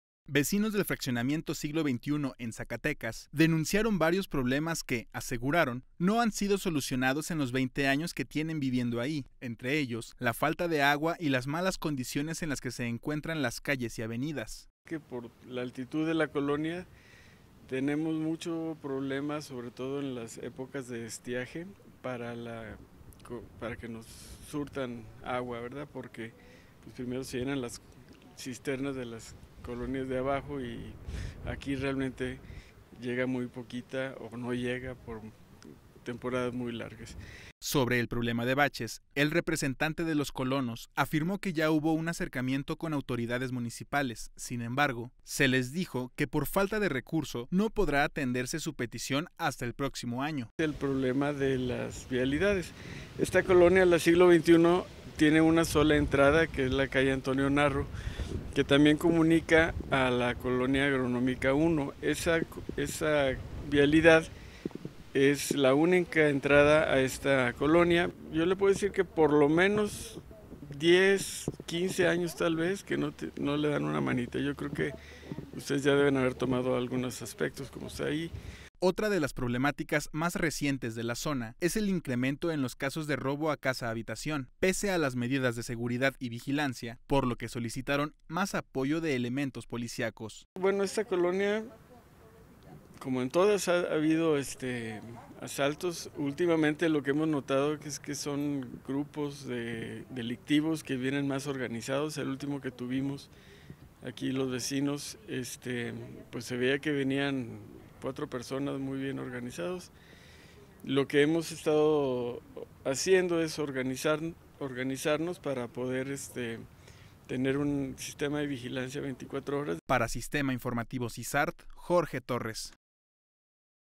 DenunciaCiudadana SizartSíTeEscucha
Vecinos del fraccionamiento Siglo XXI, en Zacatecas, denunciaron varios problemas que, aseguran, no han sido solucionados en los 20 años que tienen viviendo ahí.